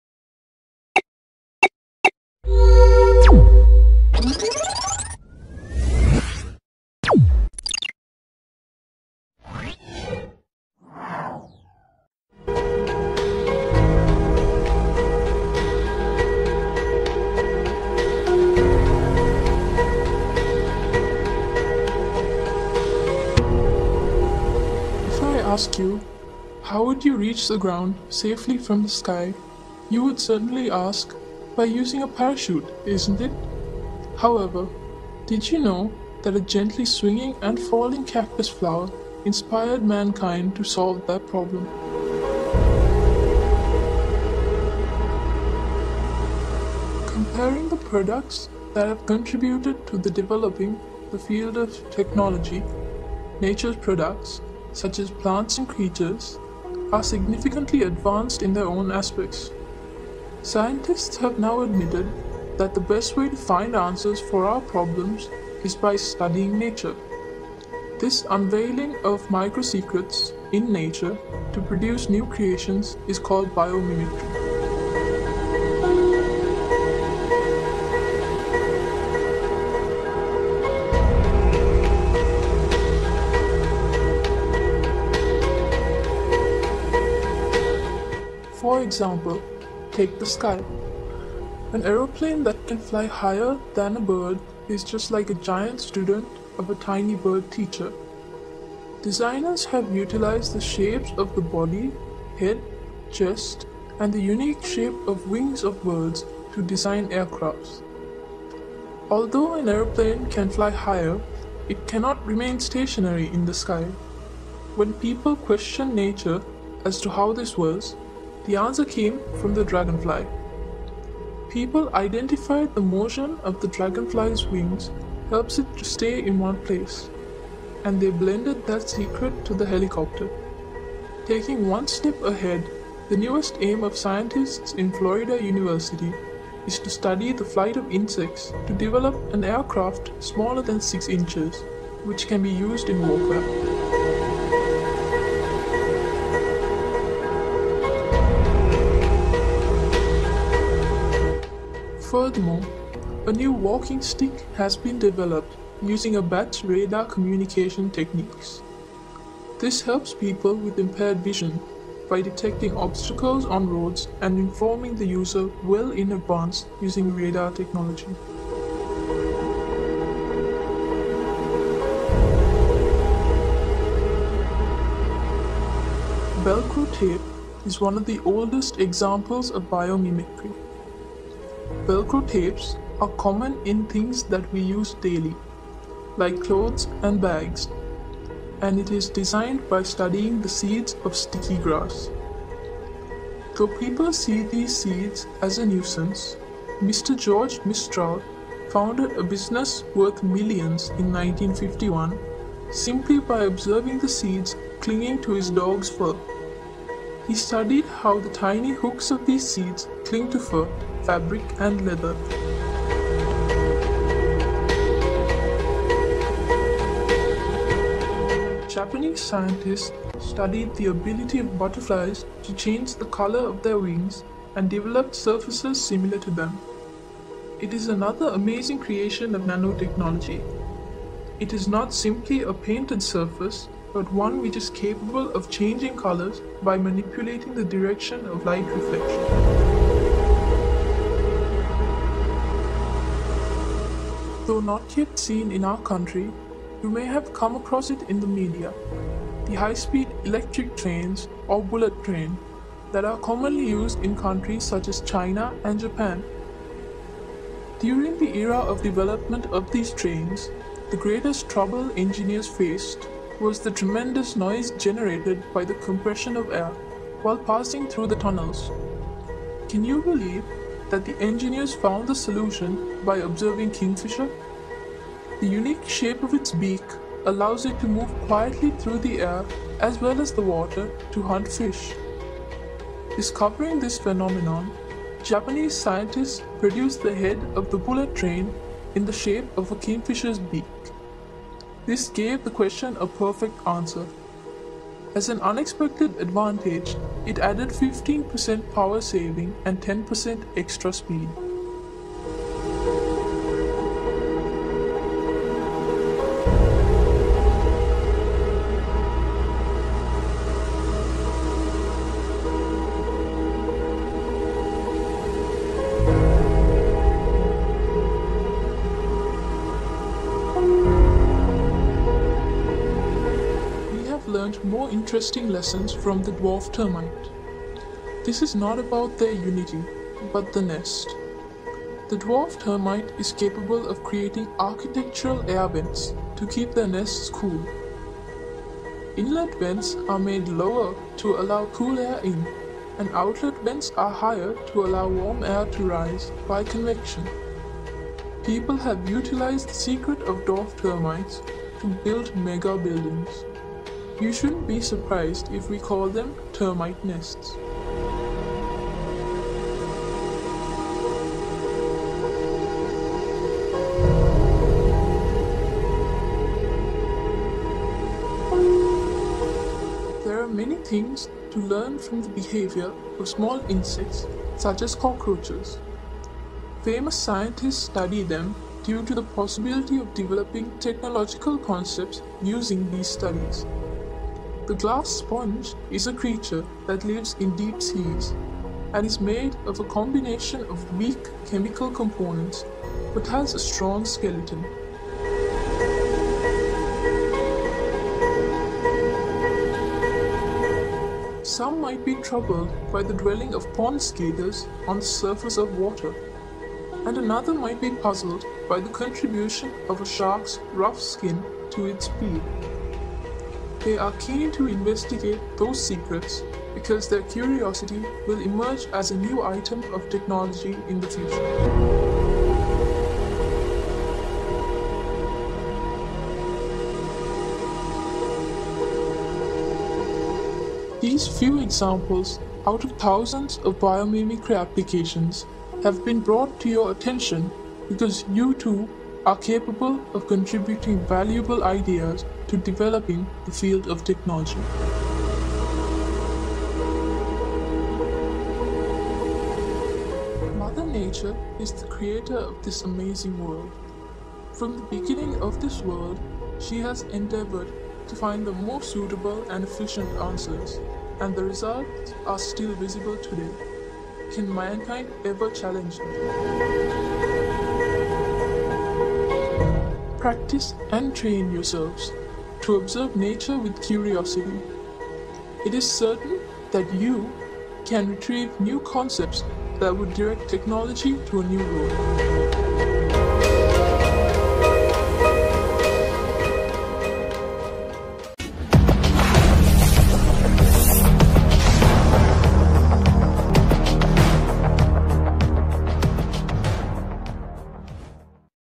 How Science is copying nature to build technology.
In this video, you'll get to know about various technologies copied from nature.